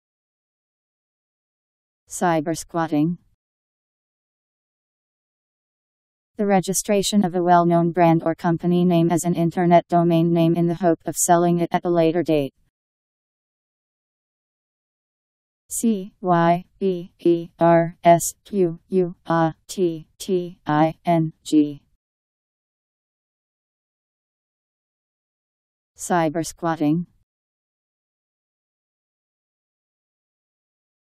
Cybersquatting Meaning
Video shows what cybersquatting means. The registration of a well-known brand or company name as an Internet domain name in the hope of selling it at a later date.  Cybersquatting Meaning. How to pronounce, definition audio dictionary. How to say cybersquatting. Powered by MaryTTS, Wiktionary